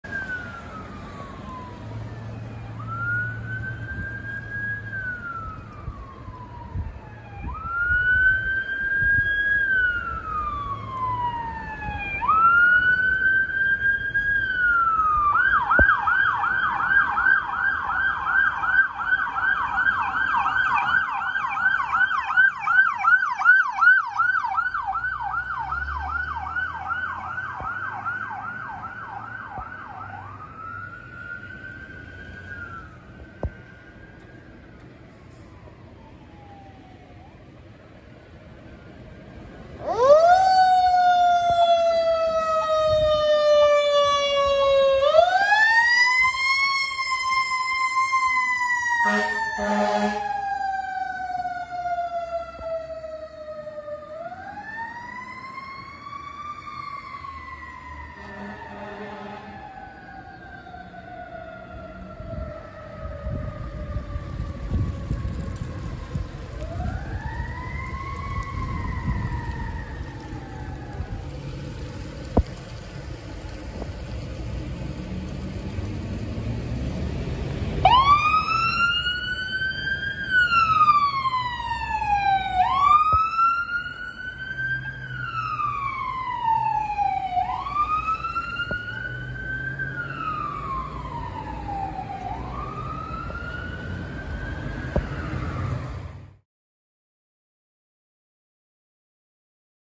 Thomaston Engine 3, Utility 5, & Rockland Rescue 1 Responding to MVA
Thomaston Engine 3, Utility 5, and Rockland Rescue 1 responding to a MVA at the New County/Dexter St. intersection.  From what I understand, Thomaston Ambulance crew was using one of Rockland's ambulances, as theirs was out-of-service at the time.  Shortly before Engine 3 arrived, a unit on-scene told dispatch that all fire units could come in non-emergency as it was a minor crash.  Taken: 11/19/2020.